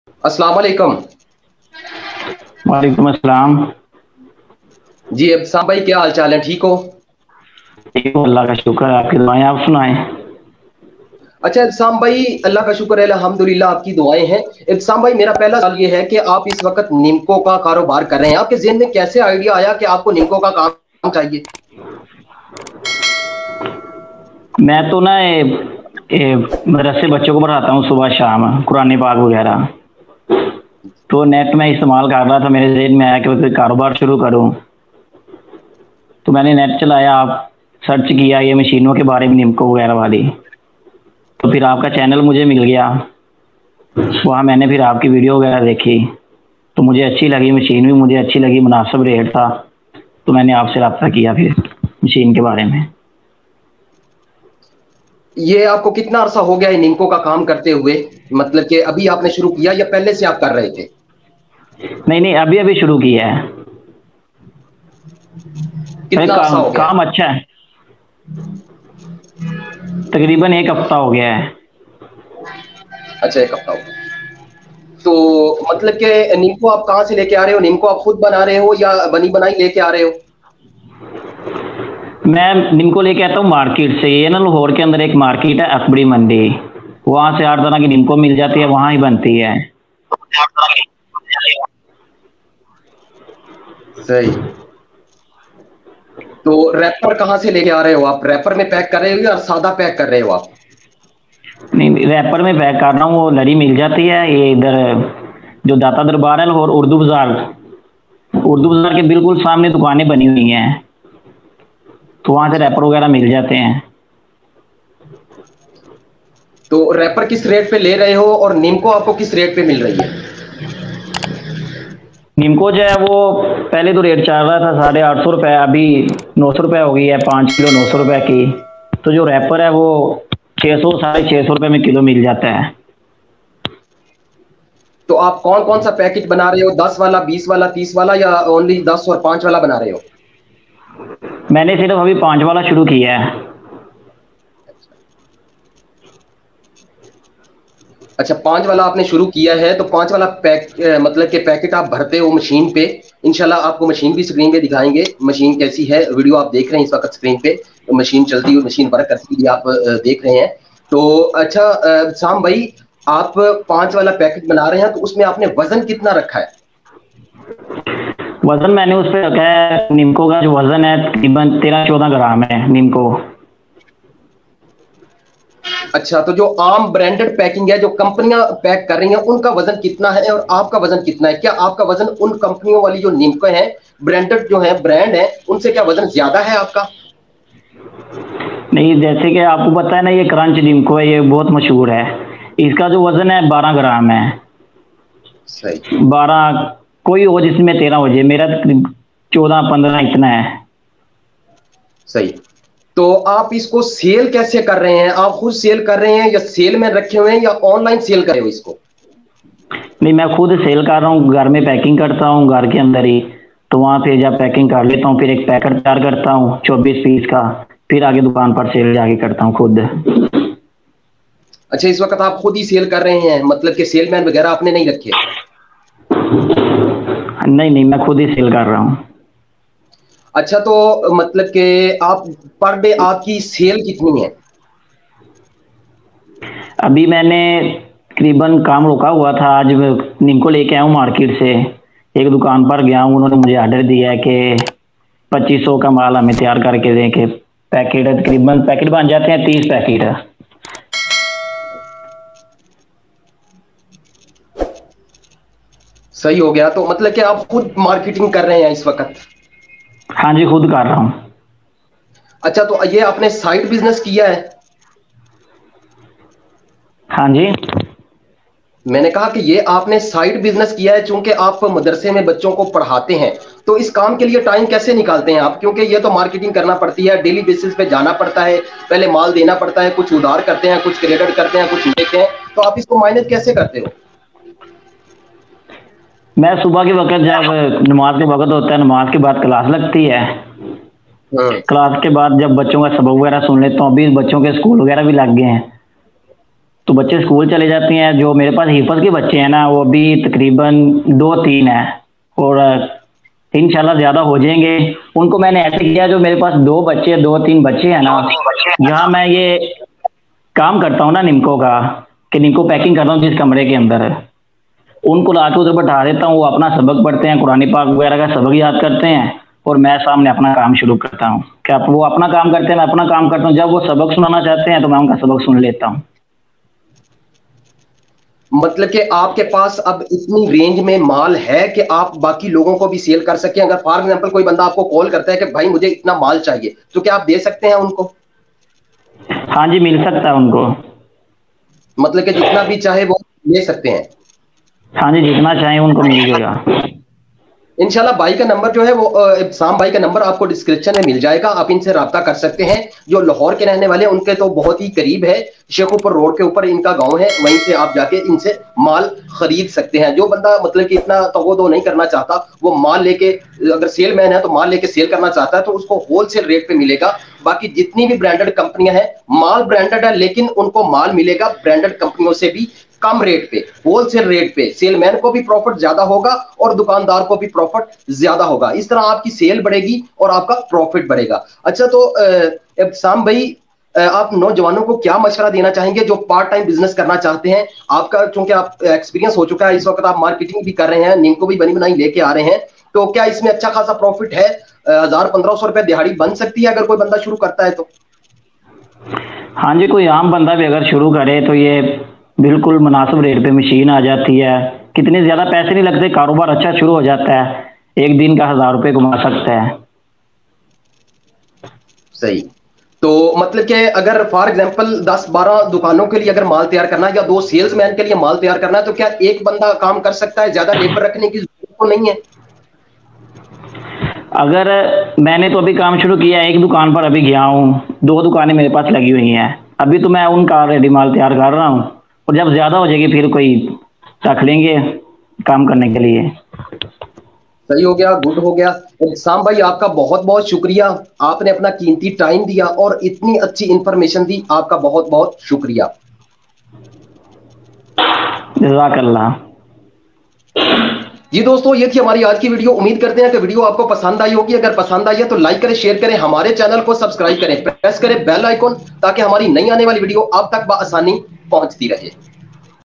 how to start nimko business dily earn 2000 hindi urdu نمکو کا کاروبار مکمل معلومات
نمکو کا کاروبار شروع کریں پچاس ہزار روپے لگائے اور ڈیلی کمائی ایک ہزار روپے خواتین اور سٹوڈنٹ بھی یہ کام کر سکتے ہیں پارٹ ٹائم بزنس آئیڈیا